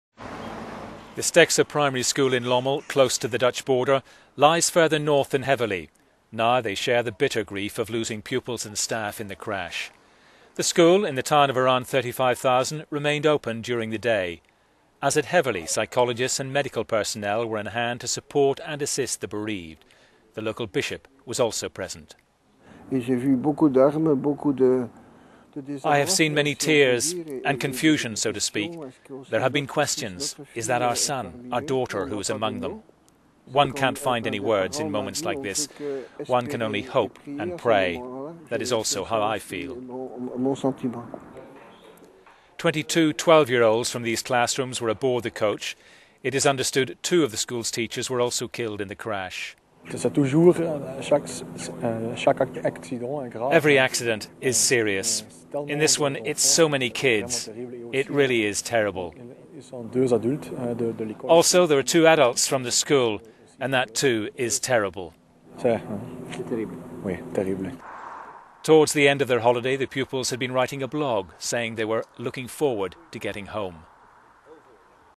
A town in mourning for its children
The Stekske primary school in Lommel, close to the Dutch border, lies further north than St. Lambertus in Heverlee. Now they share the bitter grief of losing pupils and staff in the crash.

The school in the town of around 35,000 remained open during the day. As at Heverlee psychologists and medical personnel were on hand to support and assist the bereaved. The Bishop of Hasselt, Monsignor Patrick Hoojmaertens was also present.

"I have seen many tears and confusion, so to speak. There have been questions, is it that our son, our daughter who is among them?  One can't find any words, in moments like this one can only hope and pray. That is also how I feel," he said.

Twenty two 12-year-olds from Stekske's classrooms were aboard the coach. It is understood two of the  teachers from the school were also killed in the crash as Kris Verduynkt, Deputy Mayor of Lommel explained. "Every accident is serious. It is so many kids in this one it's really terrible. Also two adults from the school, that is terrible too."

Towards the end of their holiday the pupils had been writing a blog, saying they were, "looking forward to getting home".